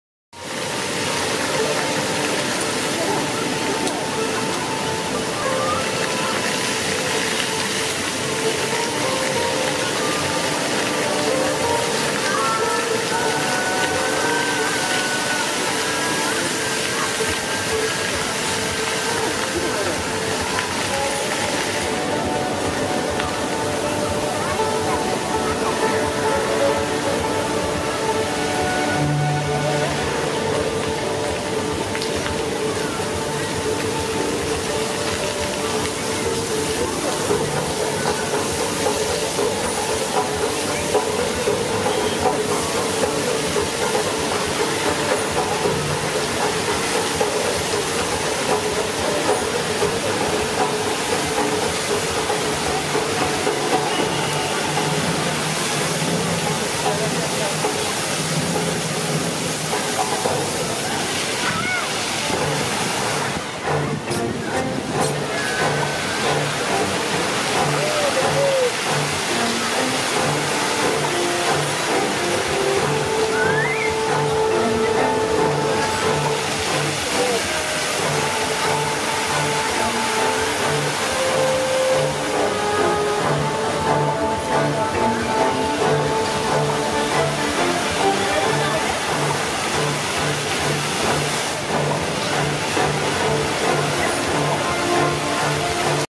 KLCC 2020 : Musical Fountain Displays Sept 2020
Fountain display at KLCC Kuala Lumpur